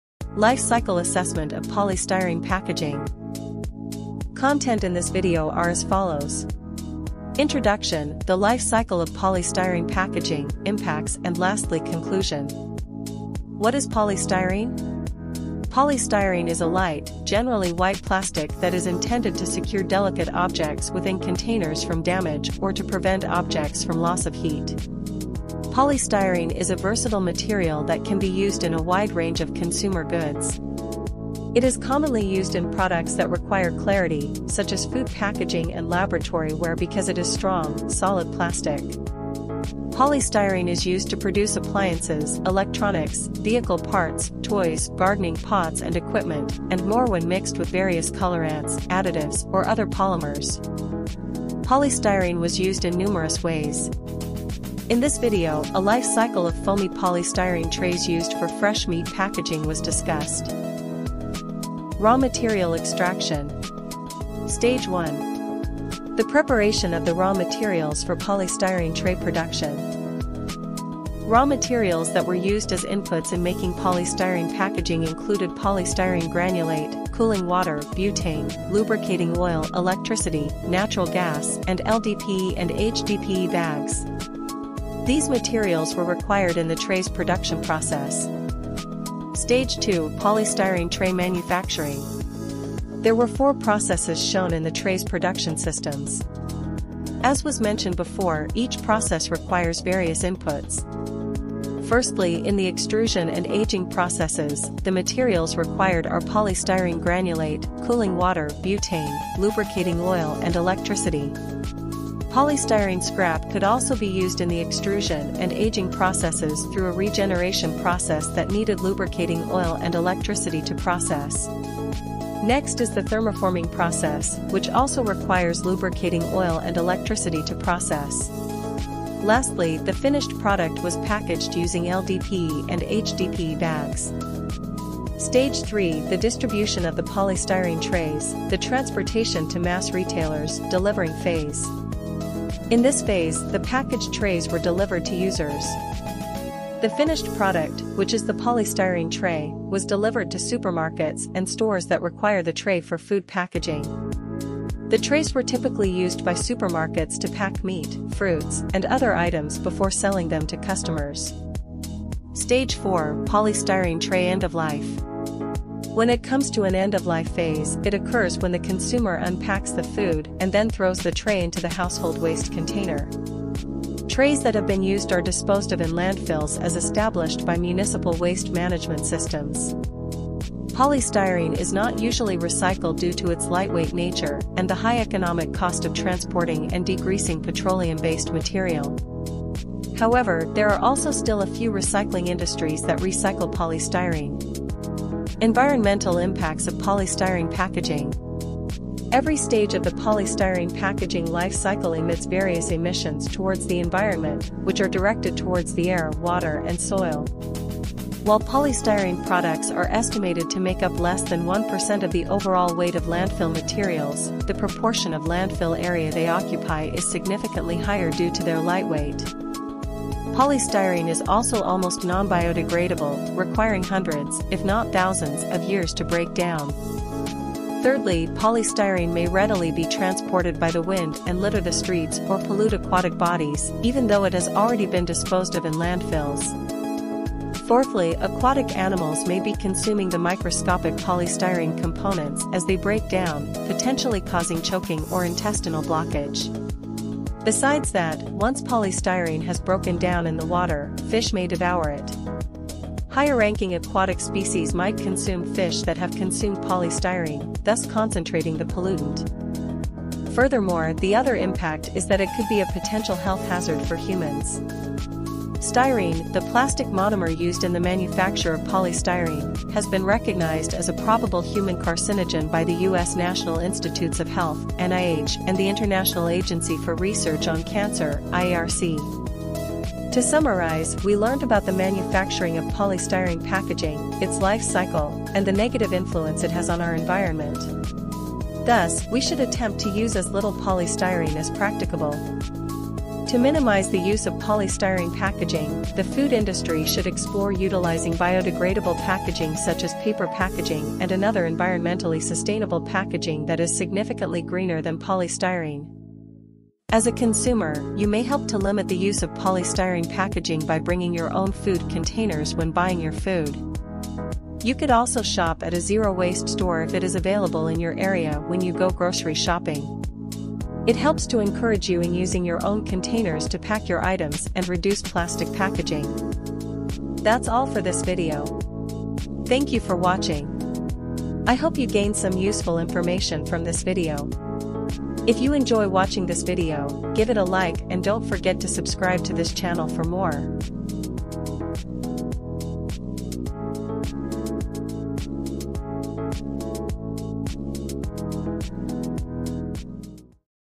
Life cycle assessment (LCA) of polystyrene packaging
Polystyrene is a versatile material that may be used in a variety of consumer products. Because of its durable characteristic, it is widely used in items such as food packaging and laboratory gear. When polystyrene is blended with various colourants, additives, or other polymers, it is used to make appliances, electronics, automotive components, toys, gardening pots and equipment, and other items. Watch the video to learn about the life cycle of polystyrene packaging.

Introduction  0:12
Life Cycles 0:54
Raw Material 1:00
Manufacturing 1:28
Distribution 2:15
Usage 2:27
End-of-life 2:42
Impacts 3:15
Conclusion 4:40